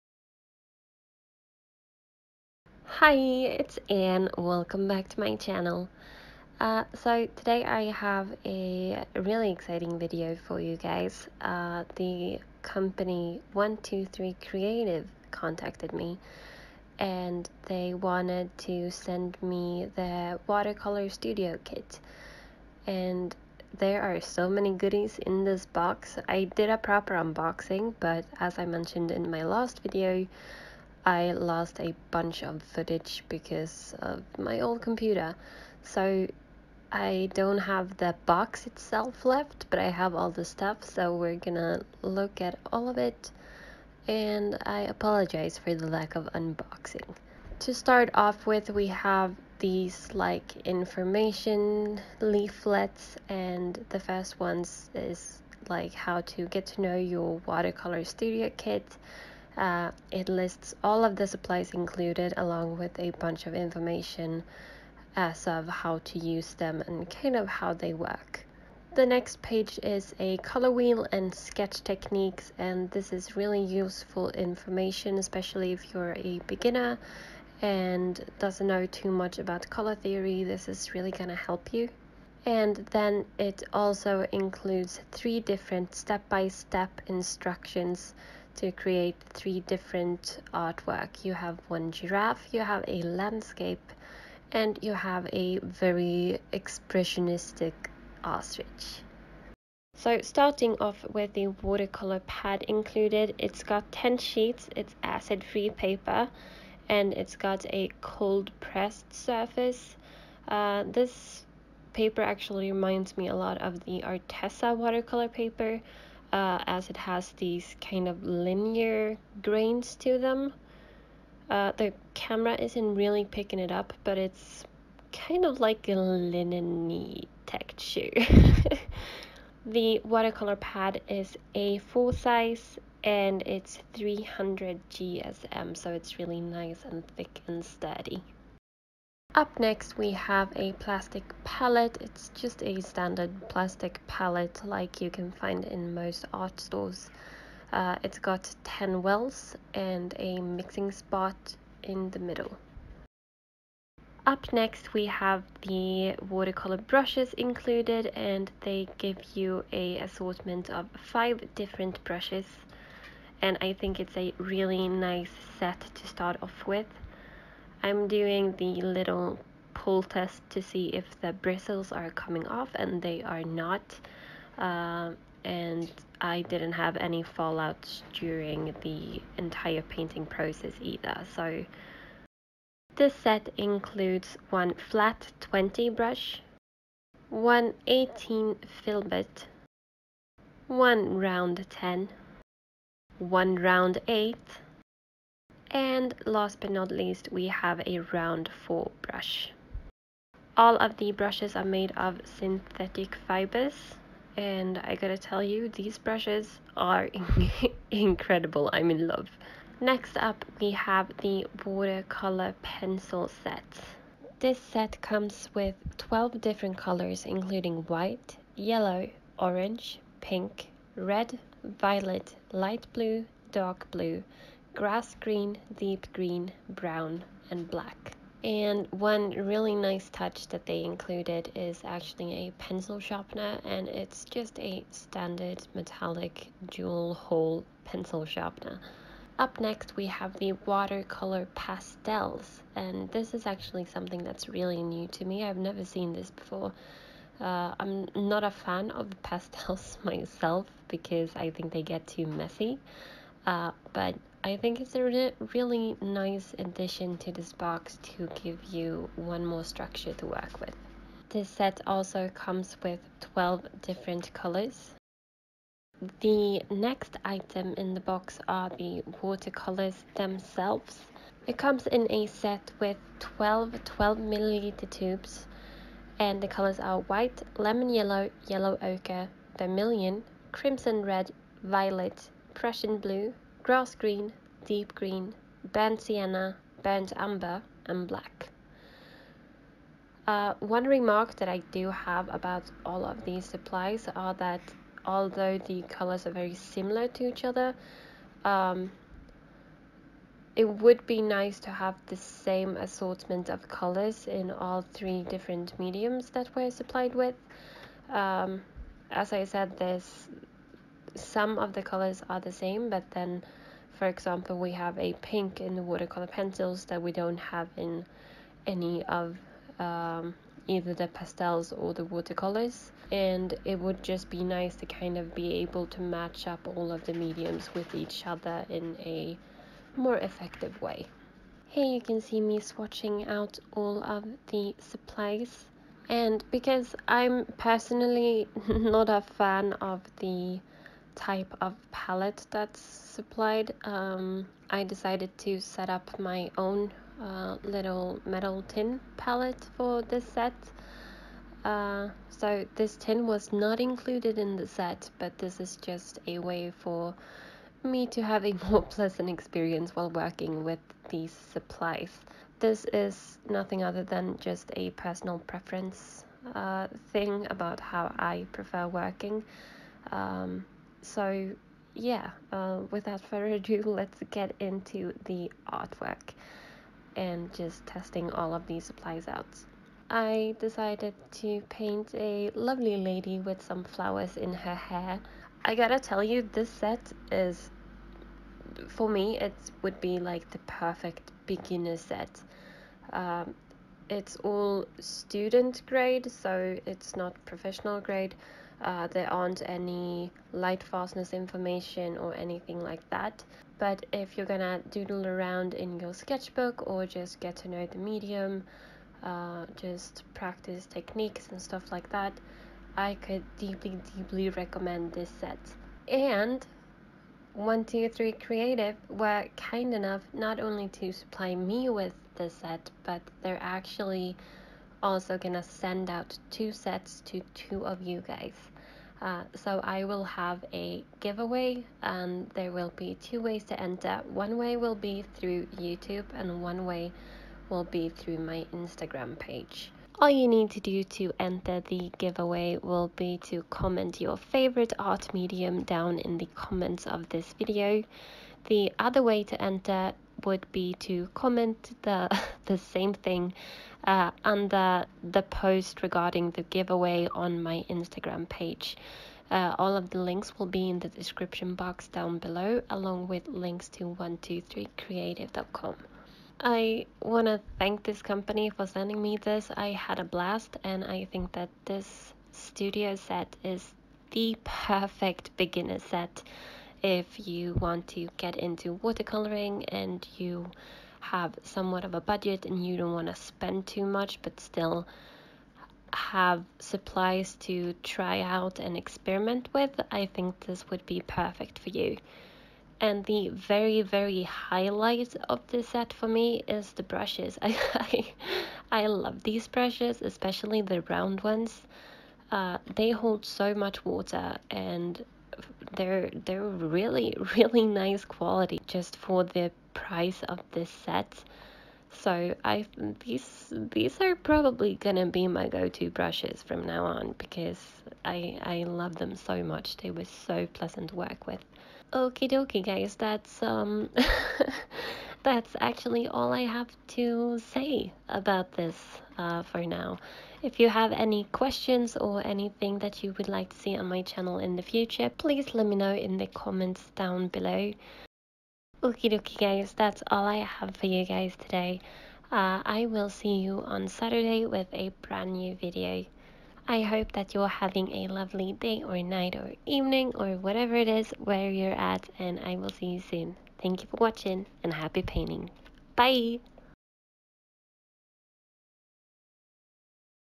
Watercolor Studio Kit GIVEAWAY and Review
So, a while back the company contacted me about doing a review on their Watercolor Studio Kit. I was so pleasantly surprised about the composition of this set, and believe it is a perfect beginners watercolor kit!

They were also kind enough to offer 2 more kits to share with you guys!

* You must be of legal age or have your parents permission to enter
* Post a comment on this video mentioning your favorite art medium and/or post a comment mentioning your favorite art medium on the giveaway post on my instagram
* Two winners will be picked randomly on March 1st 2019. One winner will be picked from YouTube and one winner from Instagram.

WHERE ELSE TO FIND ME:
➤ Etsy shop